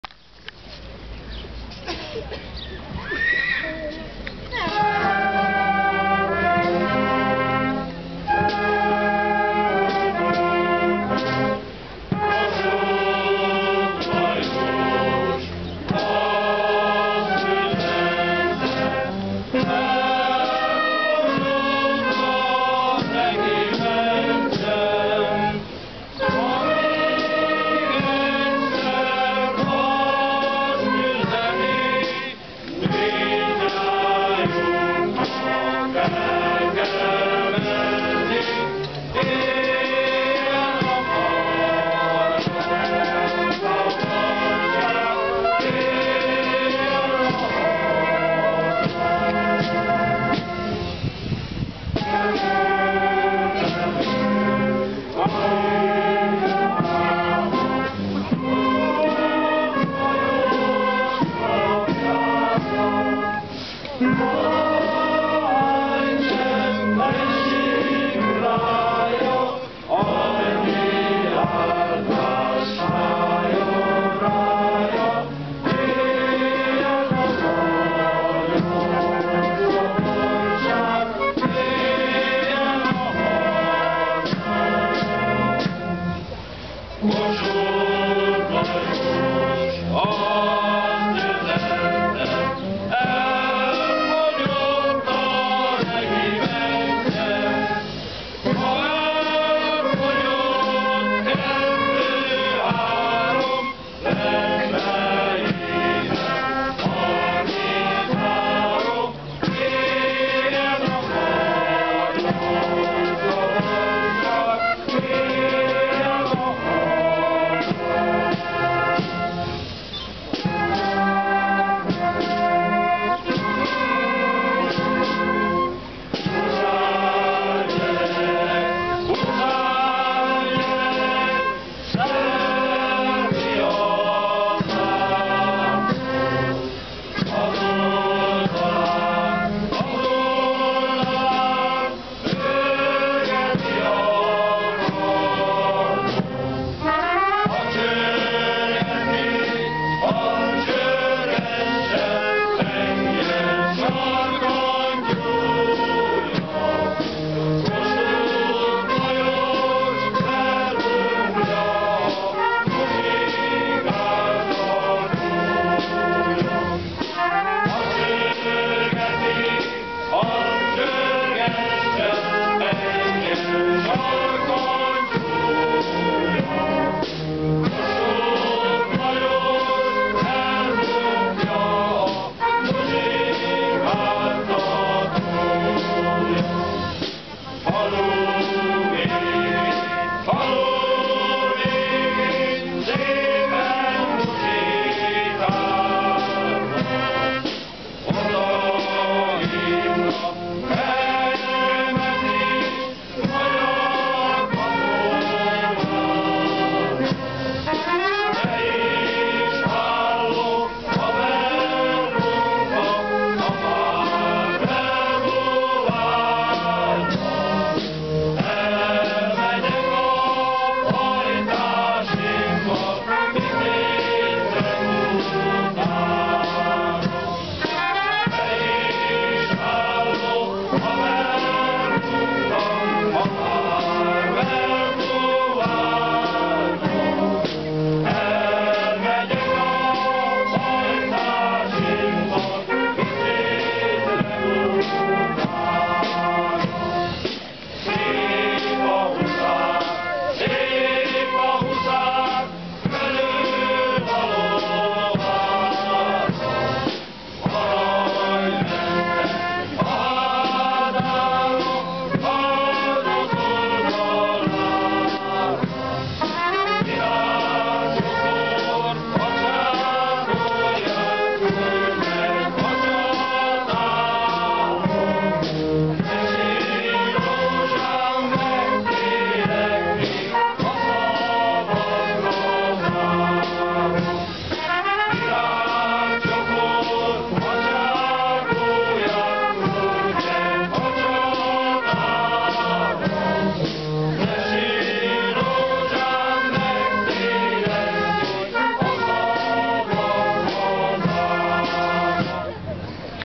Márc.15,Gyergyószentmiklós-1
Gyergyócsomafalvi fúvószenekar és gyergyószentmiklósi férfi énekkar 1848-as katonadalaokat adnak elő